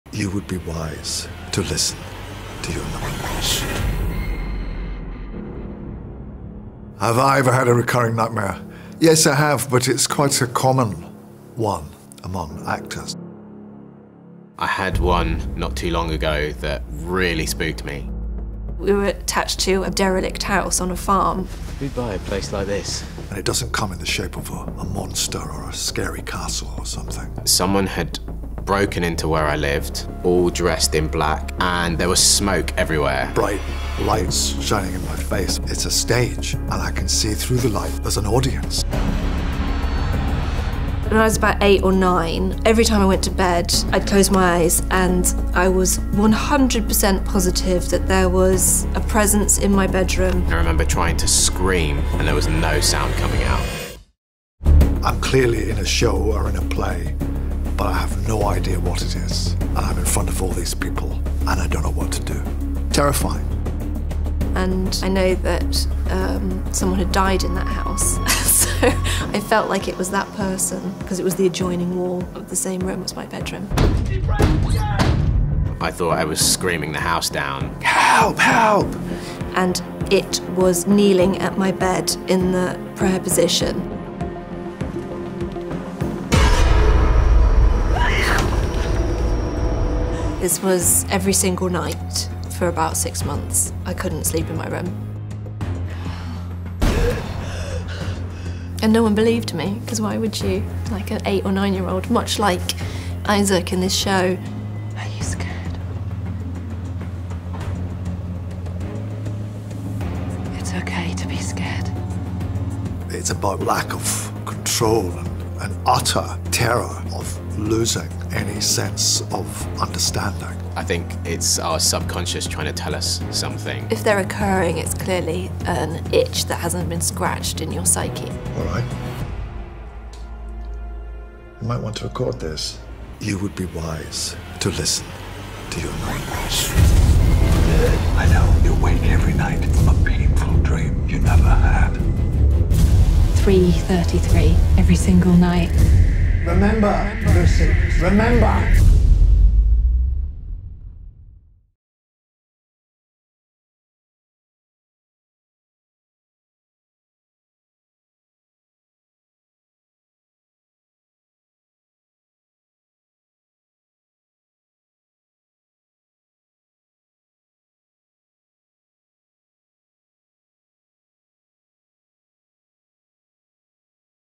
Peter Capaldi, Jessica Raine and Nikesh Patel Tell Us Their Nightmares | The Devil's Hour
The cast of The Devil's Hour reveal all in this exclusive behind-the-scenes interview. What are their recurring nightmares like? Watch The Devil's Hour on Prime Video.